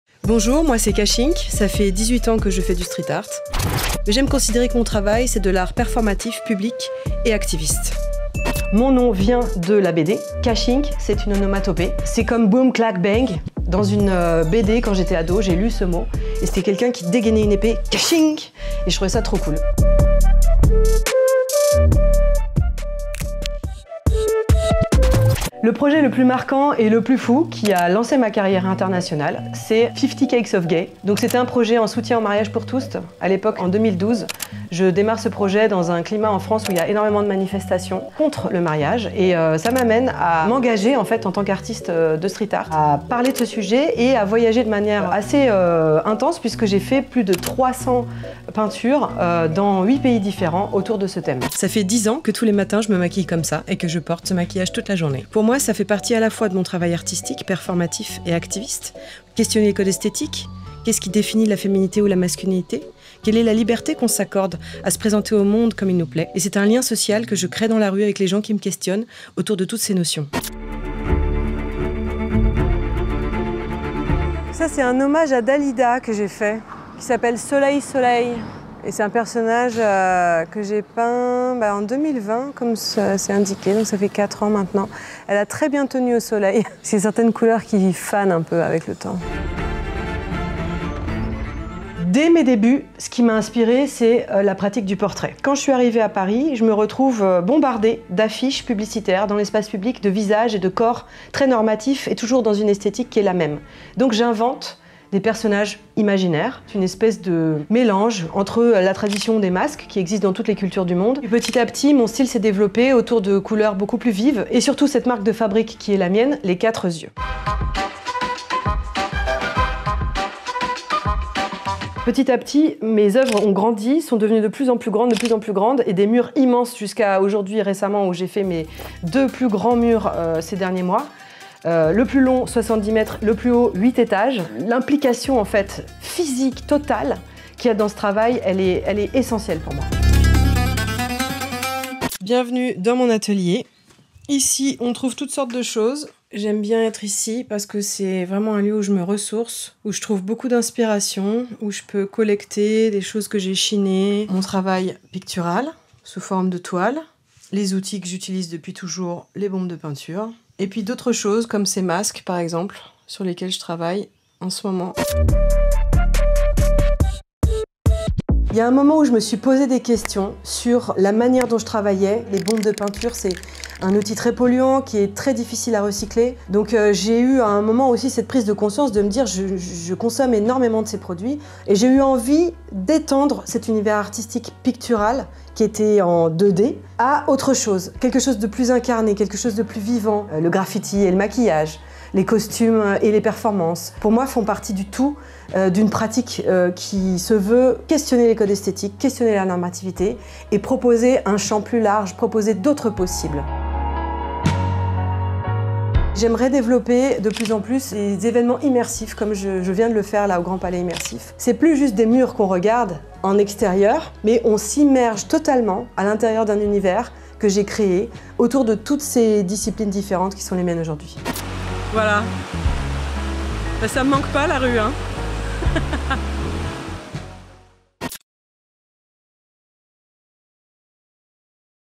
Kashink, la street artiste à la moustache - Culture Prime
Street artiste et activiste, Kashink bouscule les codes du street art avec ses fresques colorées et engagées. Portrait.

📩 Inscrivez-vous à la newsletter@cultureprimesur : cultureprime.fr